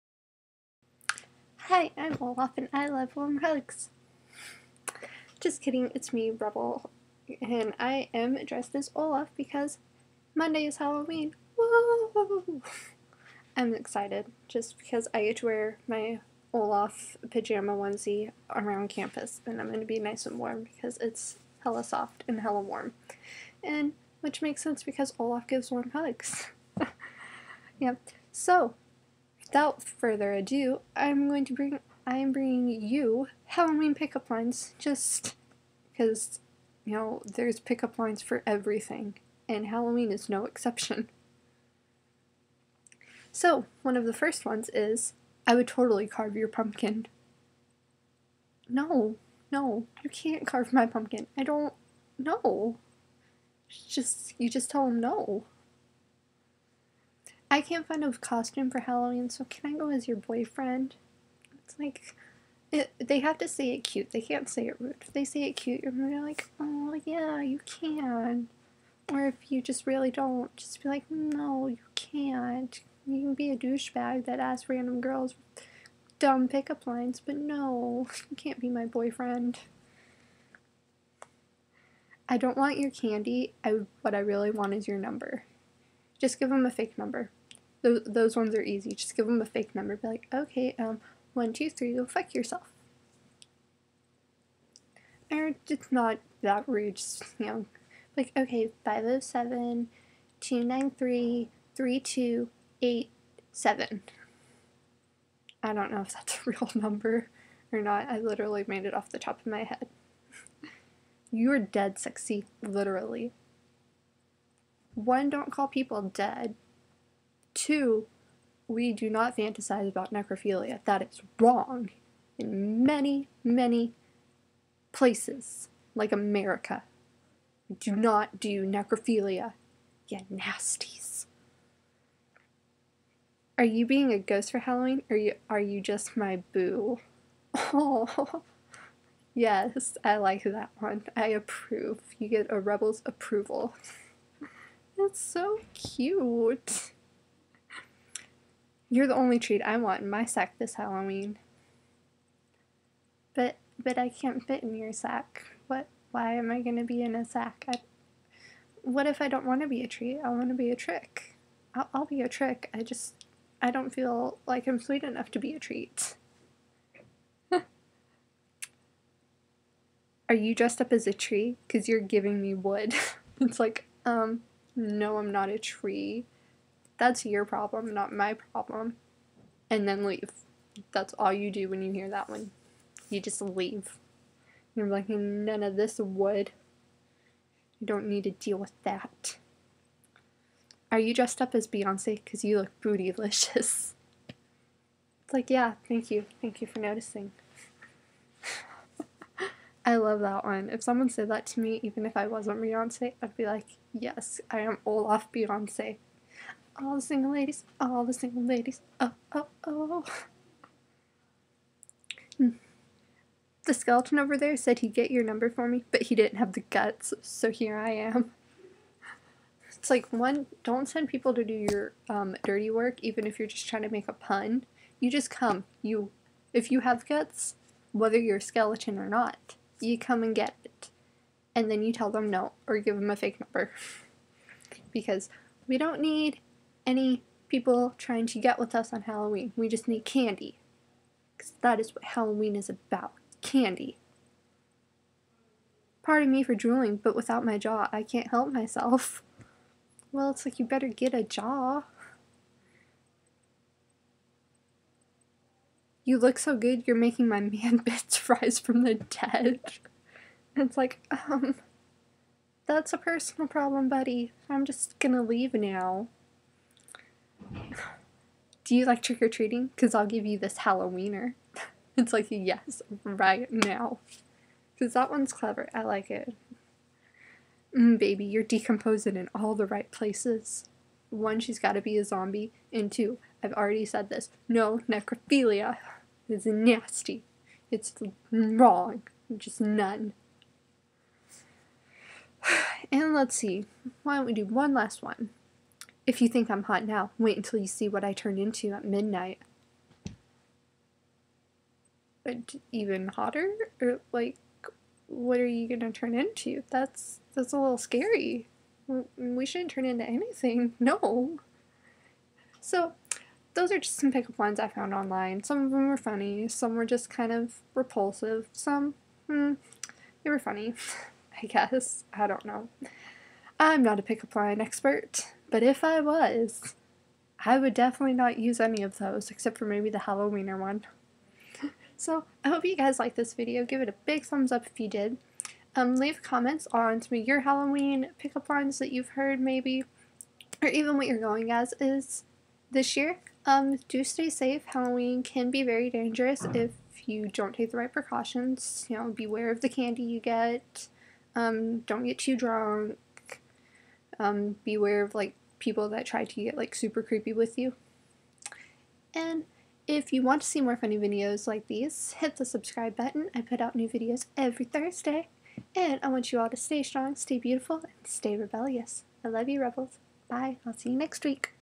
Halloween Pickup Line -What Even Are Pick Up Lines Pt 2
HAPPY HALLOWEEN REBELS!!
I hope you enjoy these crazy pick up lines and have a fun and safe halloween!